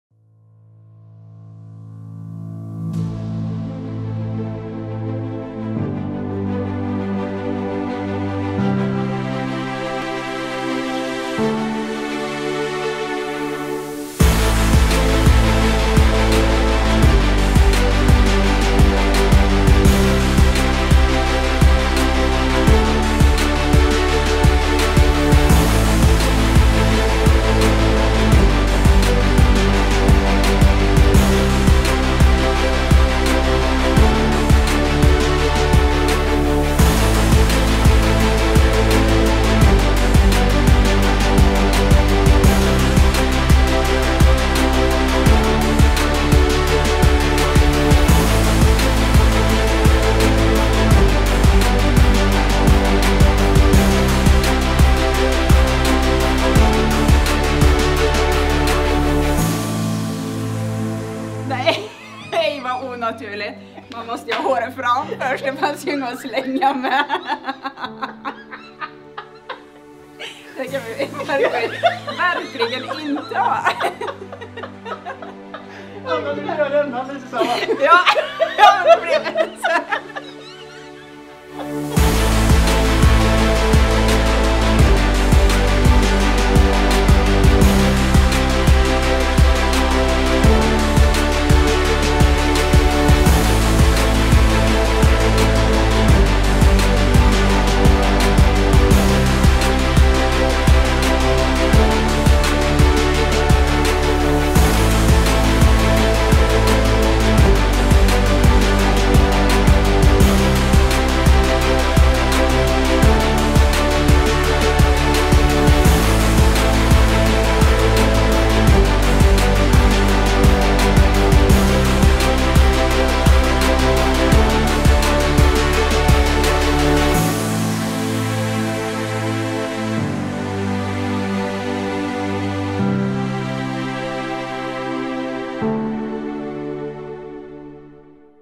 Paralympic Camp 2019
Highlights från Paralympic Camp 2019 där samtliga kandidater inför Paralympics i Tokyo 2020 samlades.